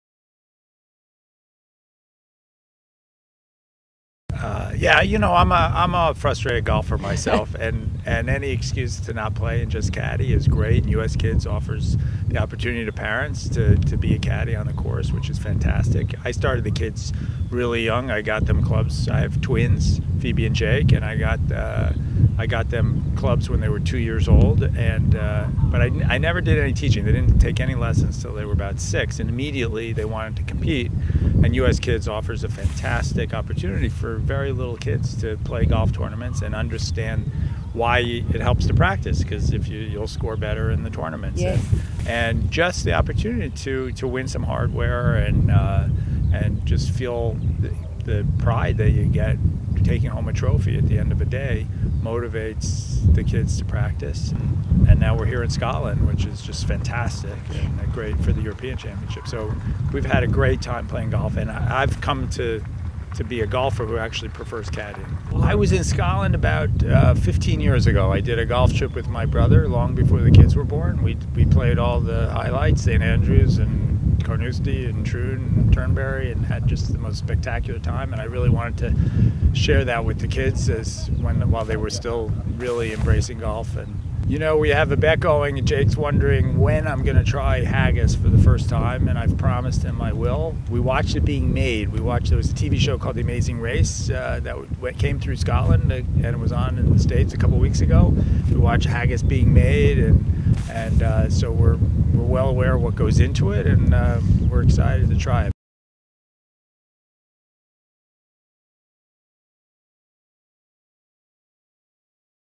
Hollywood Director Swaps Movie Sets For Golf Courses
Hollywood director, David Frankel - famed for movies such as The Devil Wears Prada - changed LA for East Lothian when he became caddy for his twin kids Jake and Phoebe at this years U.S. Kids Golf European Championship.